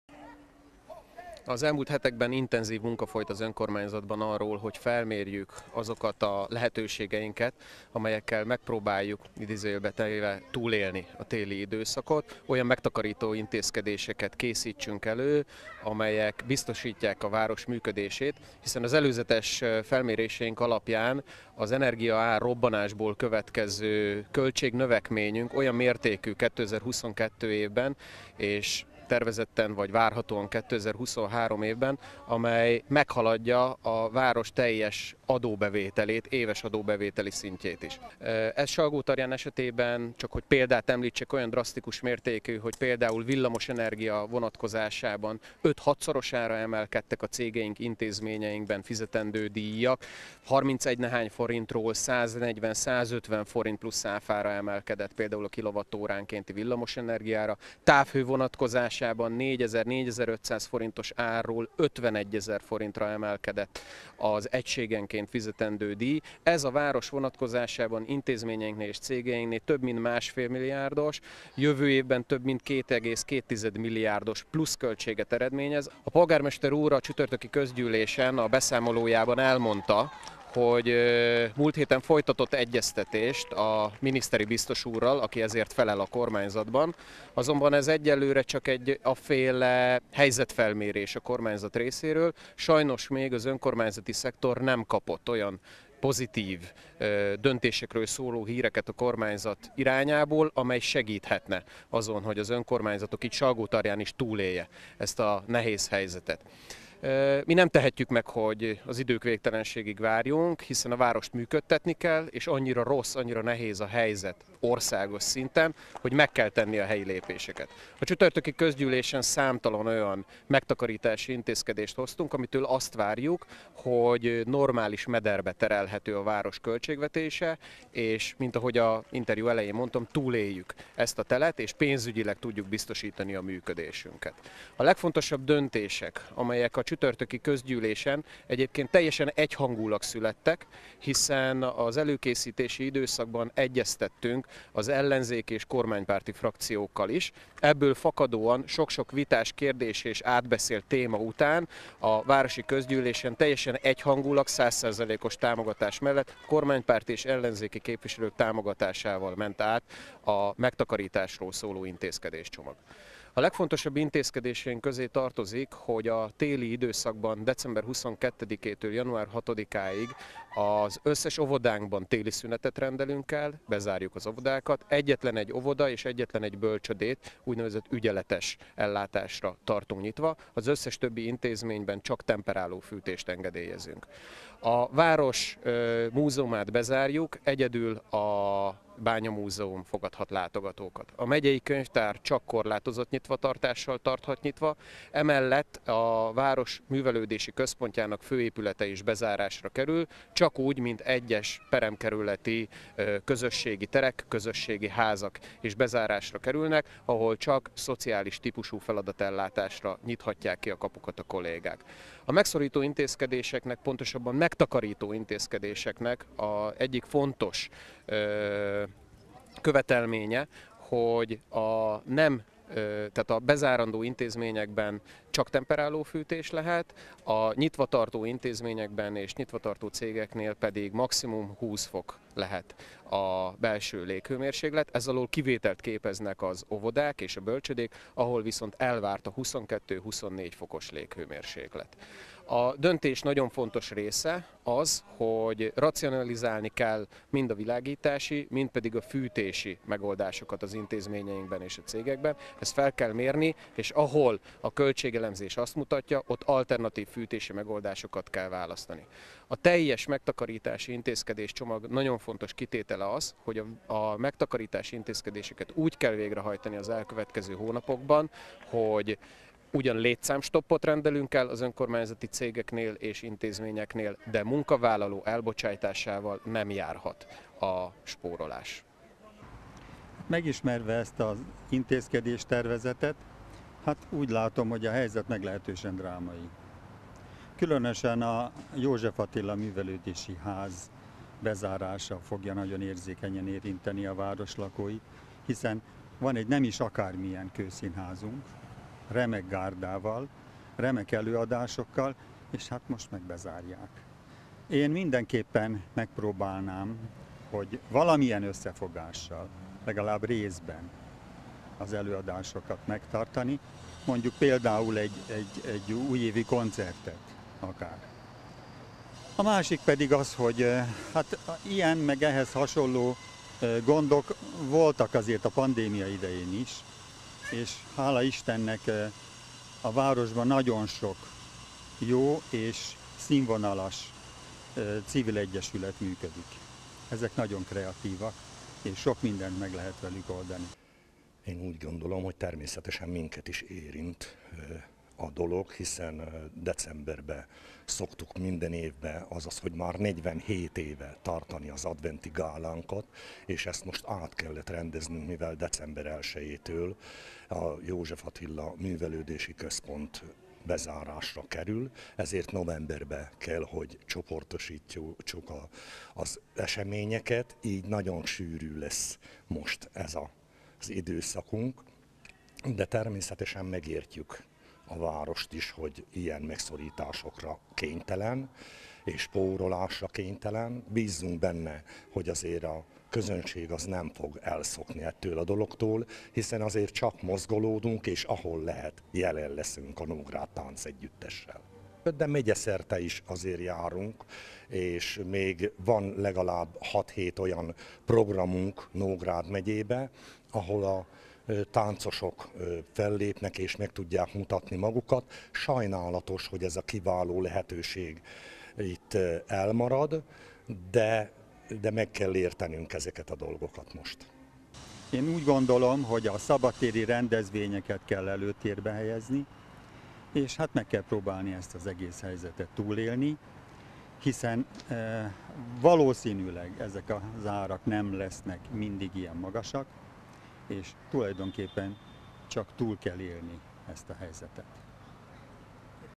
Salgótarjánban energiatakarékosság
Salgótarjánban is spórolni kell a rezsidíjak emelkedése miatt, ugyanis a városban ez idén másfél, jövőre 2,2 milliárd forintos többletkiadást jelent. Ez több, mint a nógrádi megyeszékhely teljes adóbevétele. Éppen ezért bezárnak több művelődési- és sportközpontot, és arról is döntöttek, hogy az önkormányzati fenntartású intézményekben az óvodák és bölcsődék kivételével maximum 20 fok lehet. Az ATV Híradó riportja.

A salgótarjáni önkormányzat is kész tényekkel találta magát szemben, amikor kiderült, milyen mértékben nőnek az energiaköltségek a városban. A képviselők egyhangúan döntöttek arról, hogy csak komoly megszorításokkal tudják kezelni a helyzetet.

„Olyan drasztikus mértékű, hogy például villamos energia vonatkozásában 5-6-szorosára emelkedtek a cégeinknek, intézményeinkben fizetendő díjak. Harmincegynehány forintról 140-150 forint plusz áfára emelkedett a kilowattóránkénti villamos energia ára. Távhő vonatkozásában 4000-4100 forintos árról 51 ezer forintra emelkedett az egységenként fizetendő díj” – mondta Molnár Károly, a Salgótarján Szeretem Egyesület frakcióvezetője.

Salgótarján esetében idén másfél, jövőre 2,2 milliárd forintos pluszt jelent a rezsiszámlák emelkedése. Ez több, mint az egész éves adóbevétele a városnak, így kénytelenek bezárni a tanuszodát és a művelődési központot. Utóbbi miatt sok társulat próbálni sem tud majd.

A városban élők sajnálják, hogy ilyen nehéz helyzetbe került az önkormányzat, de bíznak benne, hogy tavasztól minden visszaállhat a normális kerékvágásba.

Az önkormányzati megszorítások tavaszig lesznek érvényben. A fűtési szezon végén a most meghozott takarékossági intézkedések jó részét eltörölhetik. Közben a képviselőtestület arról is döntött, hogy a takarékosságra hivatkozva senki sem veszítheti el a munkahelyét a városnál.